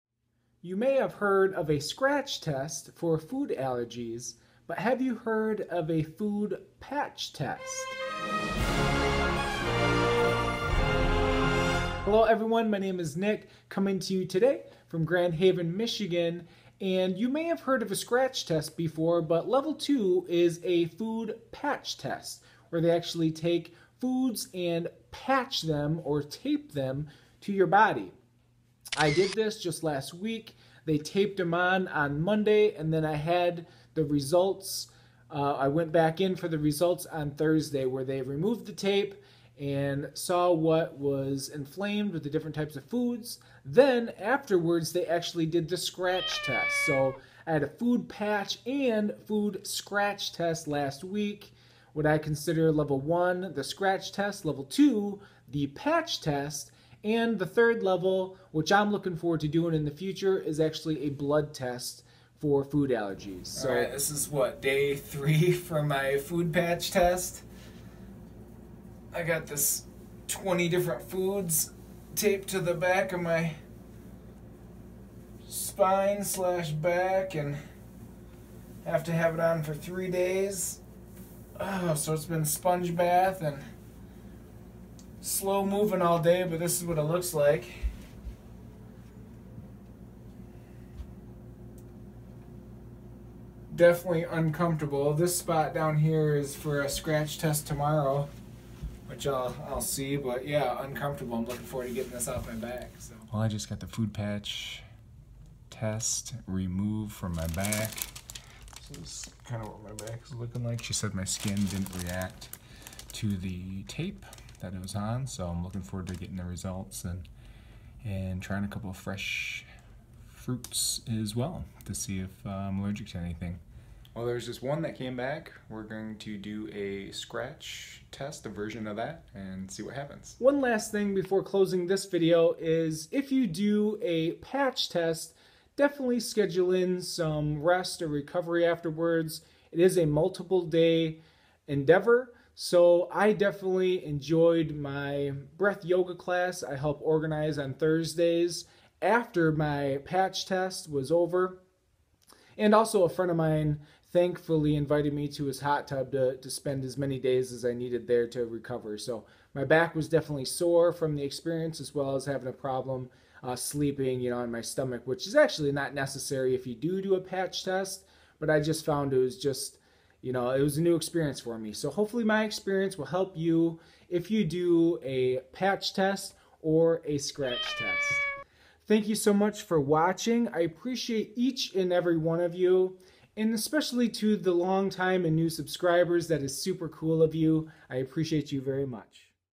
Food patch. Food allergy test
Three types of allergy tests we can get are the scratch test, patch test, and blood test to see if we are allergic to different items.  There also is some unique resonance tests I am learning about from a local doctor in Grand Haven.  In this video you will see a food patch test and a scratch test.  To learn more about a Food patch test, go

I first learned about a food patch test when researching how to enroll in a national study for my rare esophagus disease, to learn and continue growing in my health milestones.  I want to join the national study because after changing 9 items since 2008, I learned within a short 8 years, I have survived and healed myself away from the rare disease known as EoE or Eosinophilic esophagitis!

EoE is a condition not well understood, but food allergy may play a significant role.  In severe cases, my case, it may be necessary to stretch the esophagus with an endoscopy procedure.  I had mine stretched in 2008 and was told I had to come back every 6 years to get it stretched...  The doctor also said I had to take 2 prescriptions for the rest of my life.  Taking a prescription and getting my esophagus stretched for the rest of my life are unacceptable to me, so I went on a major quest for wellness that started in 2008.  I proudly learned I survived EoE in September of 2016 and now will continue to grow in my health in other areas.

To learn more about Eosinophilic esophagitis, go to this wikipedia